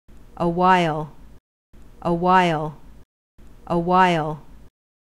How to Say or Pronounce Awhile
This video shows you how to say or pronounce Awhile.

How accurate does it say Awhile? How would you say Awhile?